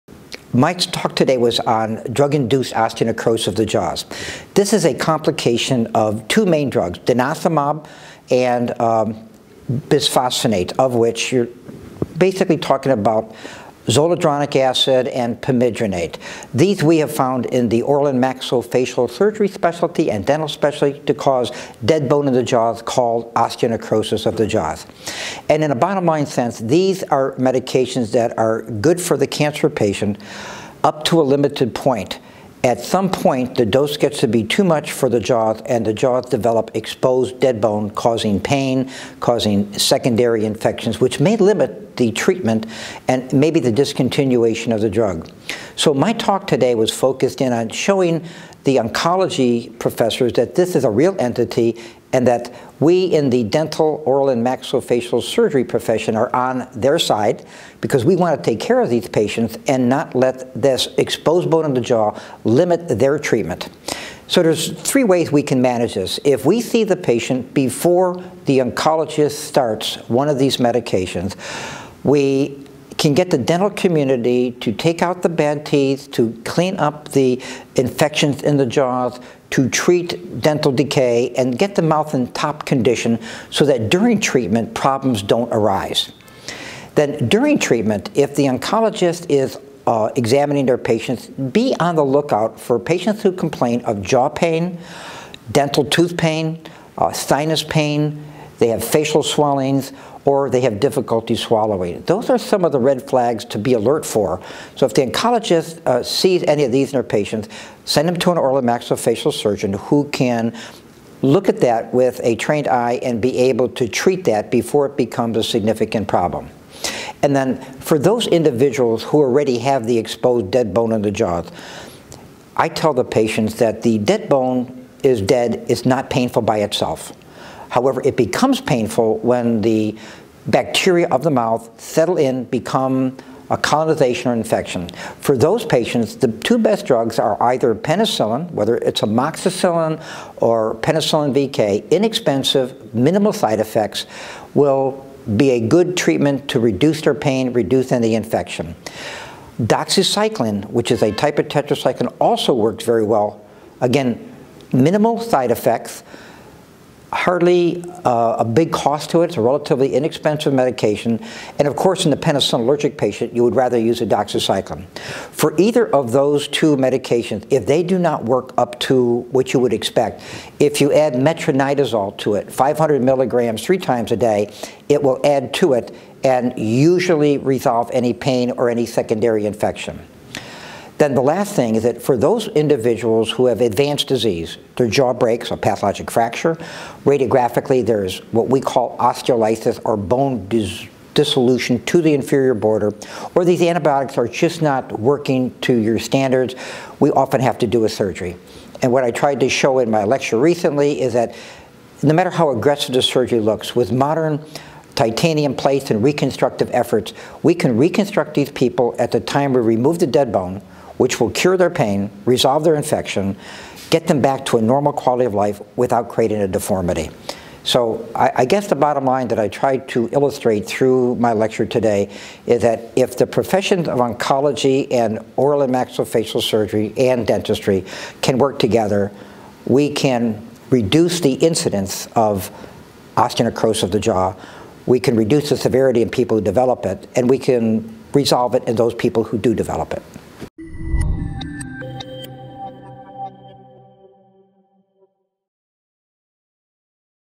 Managing osteonecrosis of the jaw
Dr Marx speaks with ecancer at NOSCM 2017 to discuss osteonecrosis of the jaw.

He outlines the cancer drugs linked to disease onset, and gives the perspective of facial/dental surgeons in minimising adverse events and promoting therapy adherence.

In terms of managing infection, Dr Marx recommends penicillin-based antibiotics or anthracyclines, and in the case of advanced disease he describes how surgical interventions can preserve appearance and quality of life.